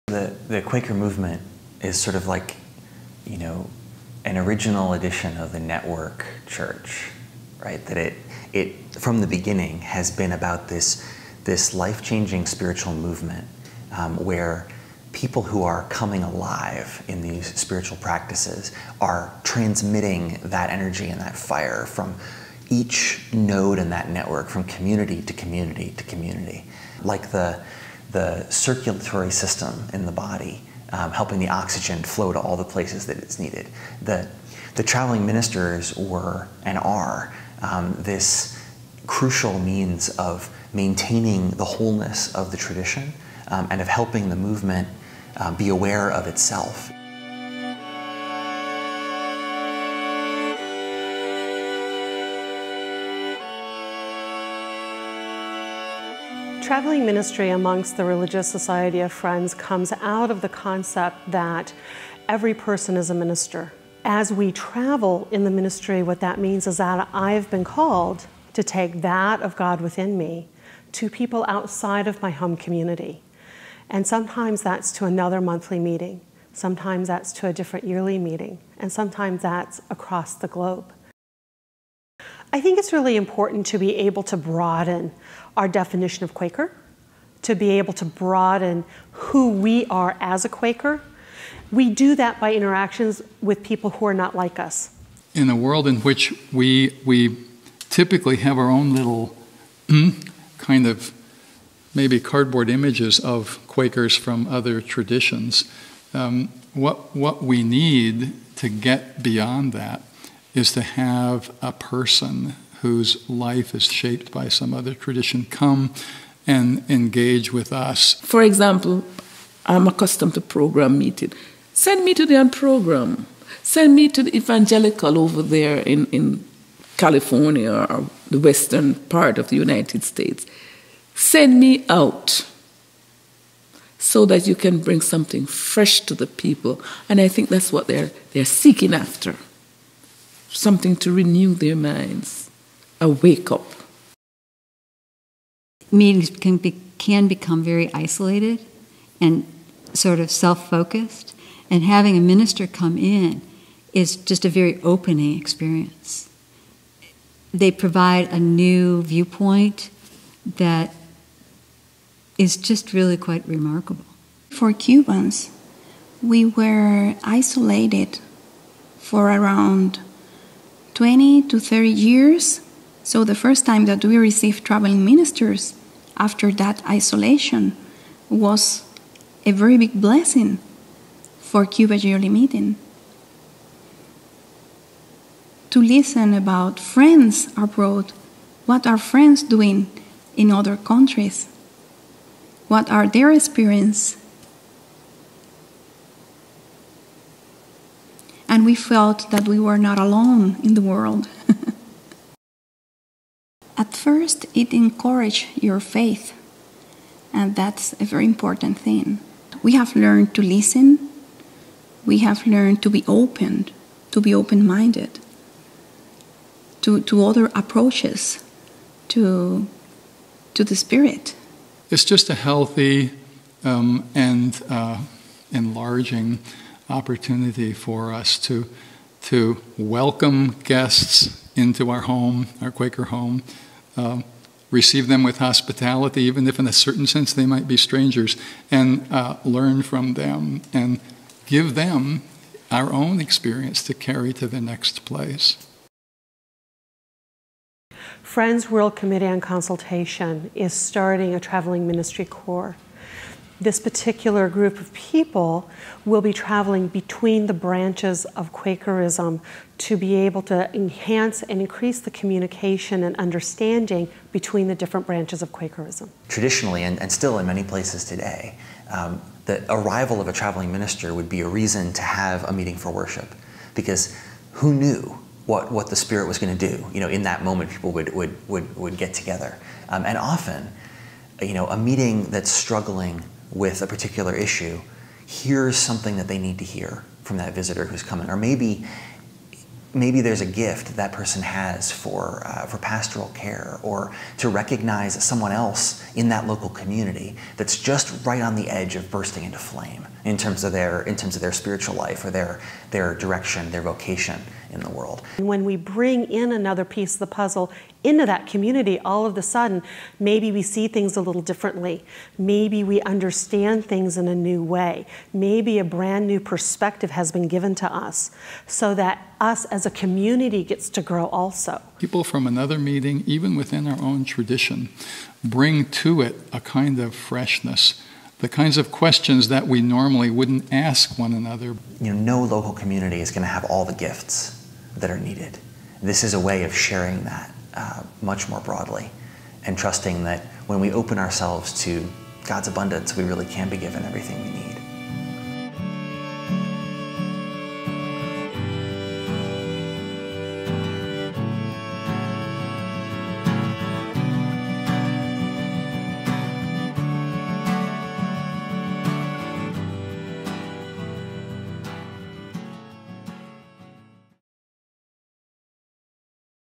Why Traveling Ministry Is Vital for Quakers in the 21st Century
In this week’s Quaker Speak, Friends explore the time-tested practice of Quaker traveling ministry, still a powerful tool for Friends meetings to “renew their minds and wake up”.

Transcript:

Noah Baker Merrill
The Quaker movement is sort of like an original edition of the network Church. From the beginning, it has been about this life-changing spiritual movement where people who are coming alive in these spiritual practices are transmitting that energy and that fire from each node in that network, from community to community to community.
Like the circulatory system in the body, helping the oxygen flow to all the places that it’s needed, the traveling ministers were, and are, this crucial means of maintaining the wholeness of the tradition and of helping the movement be aware of itself.

Kristin Olson-Kennedy
Traveling ministry amongst the religious society of Friends comes out of the concept that every person is a minister. As we travel in the ministry, what that means is that I’ve been called to take that of God within me to people outside of my home community, and sometimes that’s to another monthly meeting. Sometimes that’s to a different yearly meeting, and sometimes that’s across the globe.

Experiencing Quakerism Through Another Lens

Kristin Olson-Kennedy
I think it’s really important to be able to broaden our definition of Quaker, to be able to broaden who we are as a Quaker. We do that by interactions with people who are not like us.

The views expressed in this video are of the speakers and do not necessarily reflect the views of Friends Journal or its collaborators.